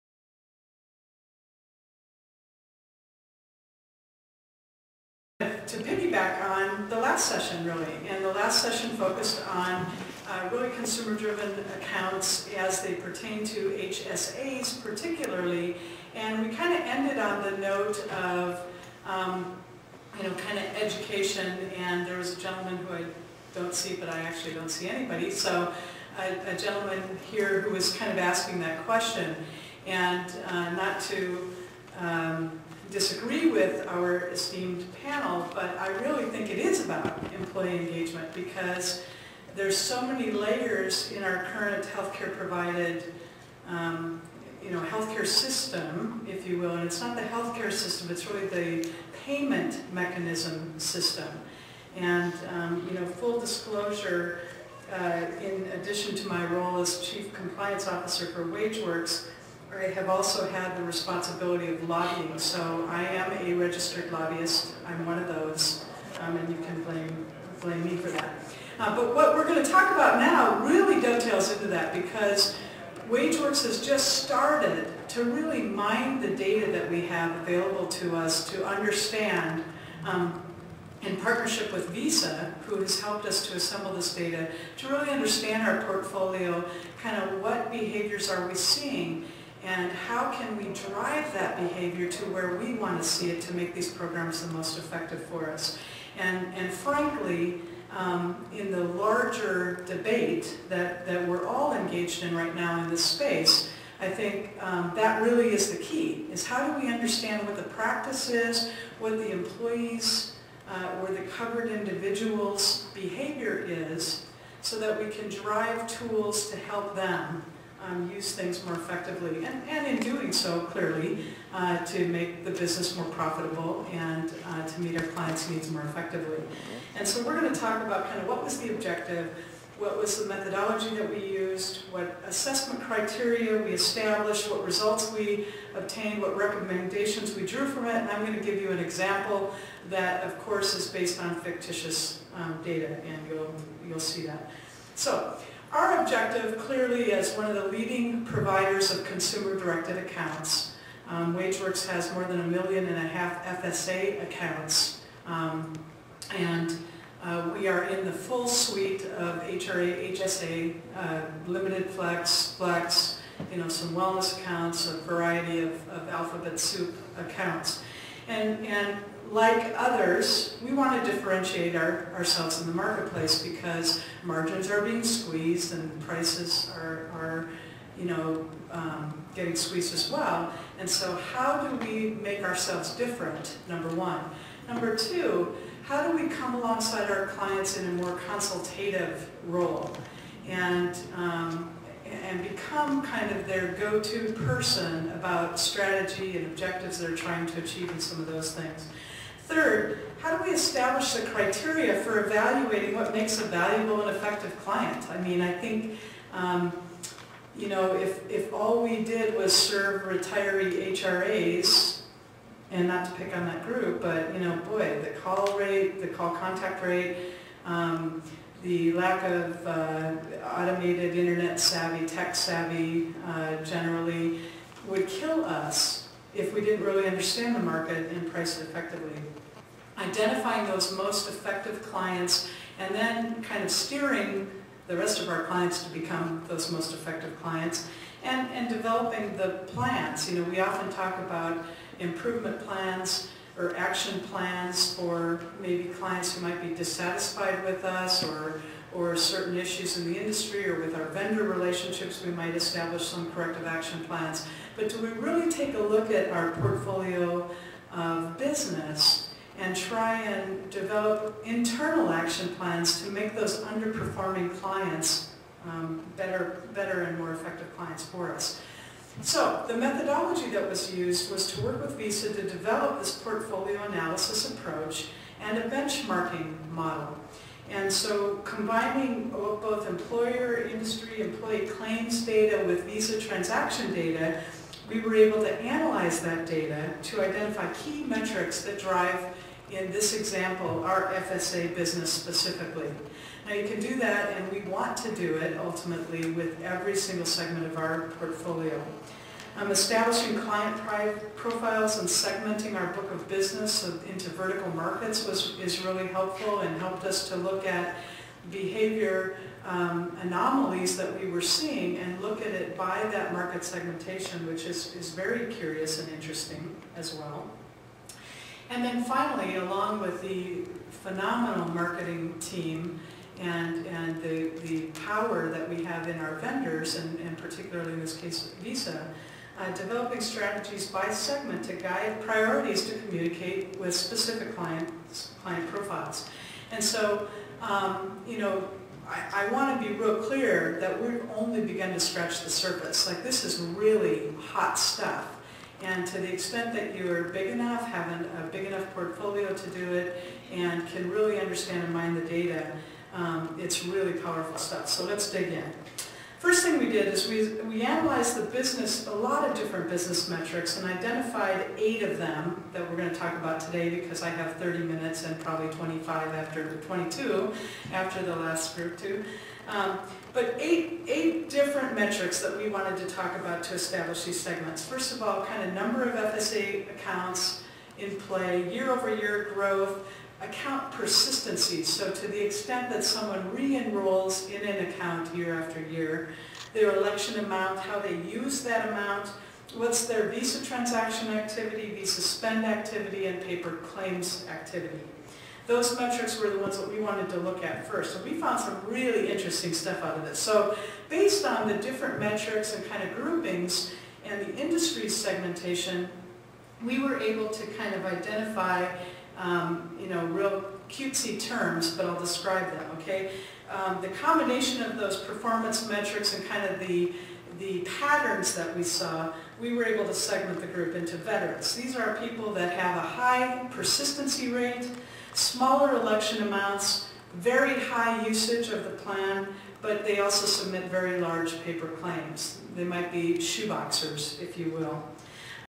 The Value of Data Analysis & Segmentation for Prepaid Healthcare Portfolio
Presenter:
Jody Dietel, Compliance Officer, WageWorks

Prepaid Expo USA 2010